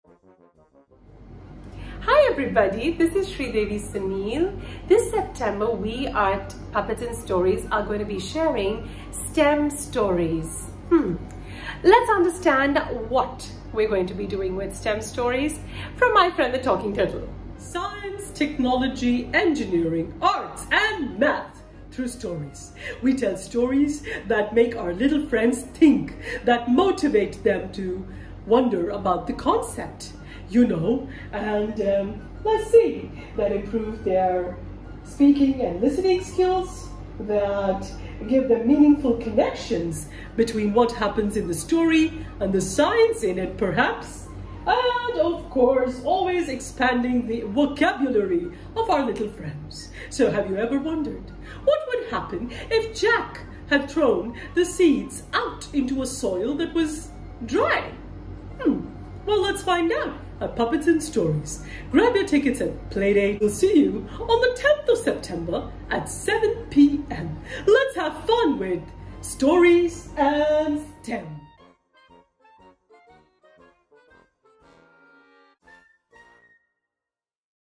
Puppets & Stories by Talking Turtles Storytellers
Puppets & Stories presents Stem Stories September
This September your puppet friends will share stories that raise curious questions!
Stories about plants, animals and the world around us !

Week1 - September 10th
Life cycle of a plant : Would Jack's beanstalk have grown if it had not been thrown out of the window and landed in garden soil?

Dates: September 10th, 17th & 24th